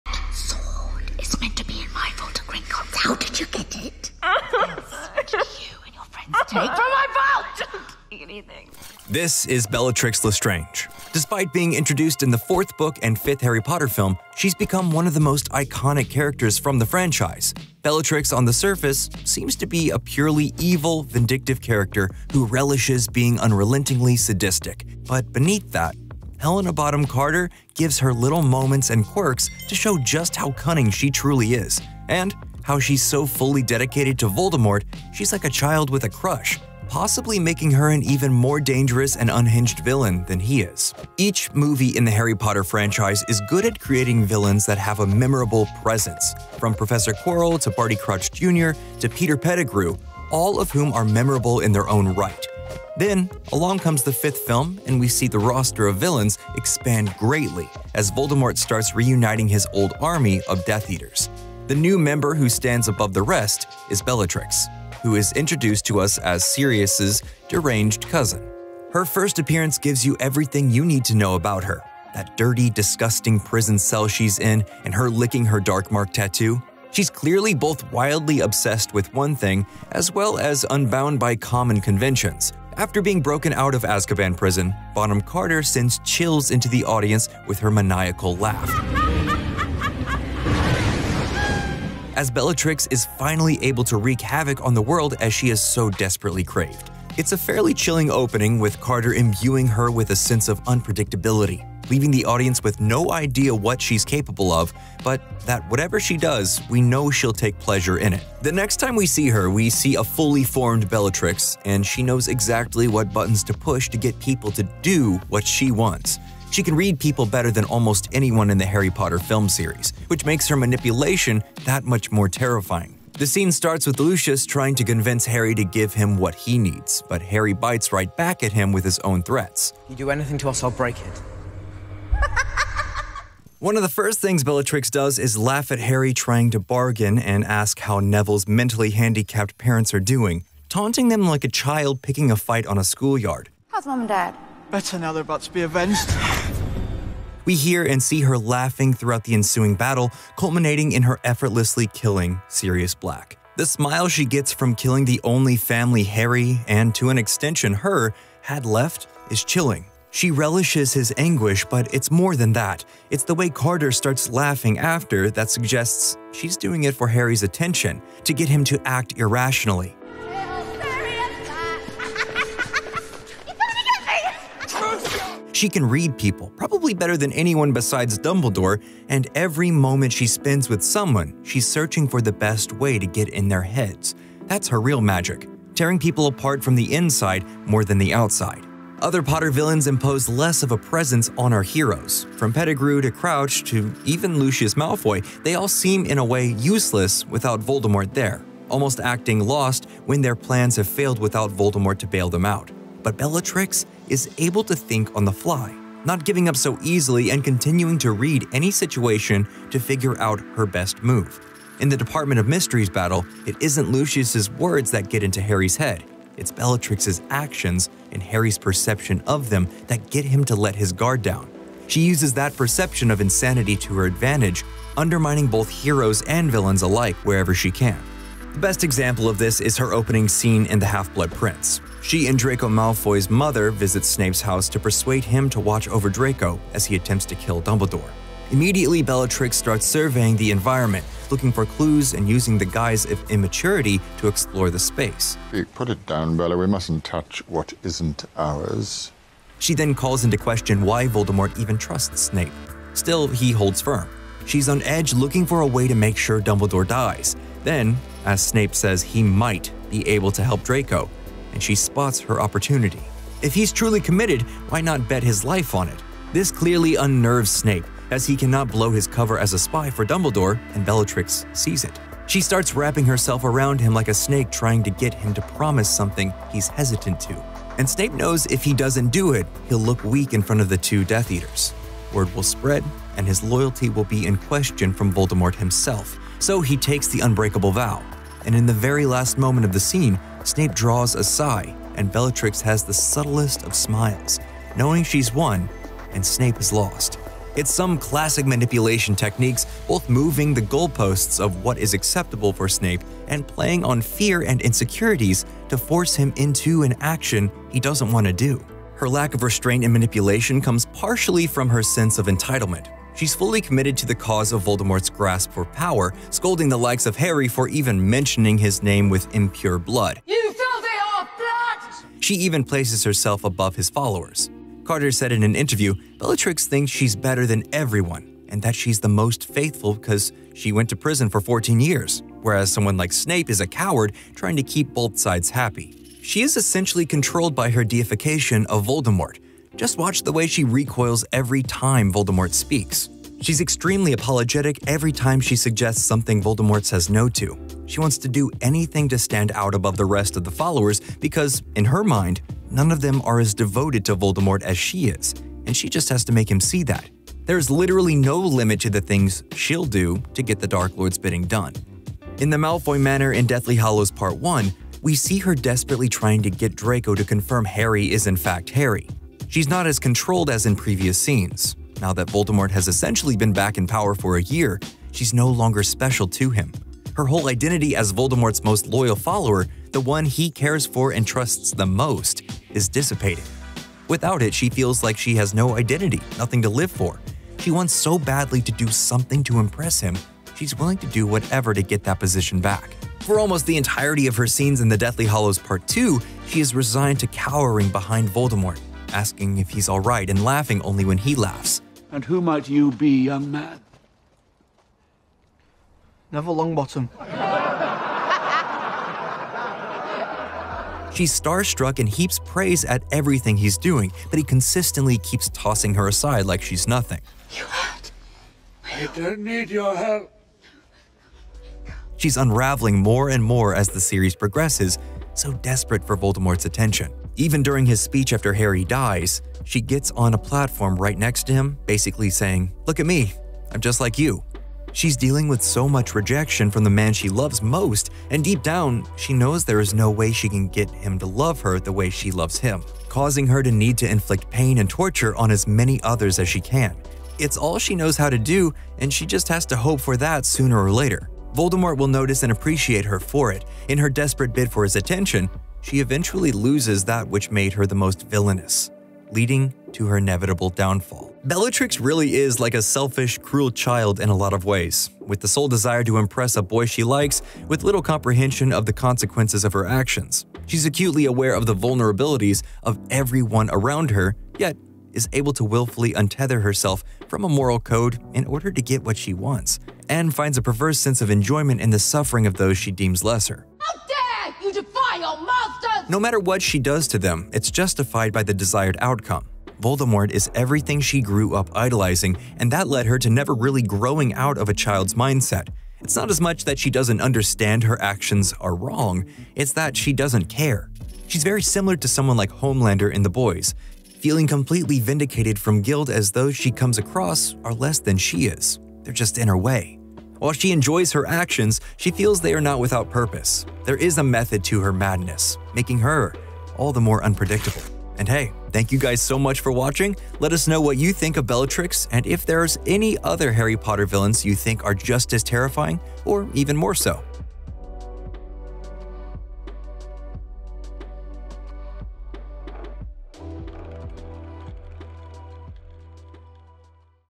What Makes Bellatrix One of The Most Terrifying Harry Potter Villains Ever
As far as Harry Potter villains go, Bellatrix Lestrange is one of the most terrifying.  Her love and admiration for Voldemort pushes her to places very few Harry Potter villains go to.  It's not about power, overtaking Voldemort, it's about doing whatever she thinks will get his attention like a little girl with a big crush.  Her actions are chaotic and psychotic, and the performance by Helena Bonham Carter puts Bellatrix Lestrange into a whole new category of Harry Potter villains.

Written by Adam Smith
Edited by David Sadvari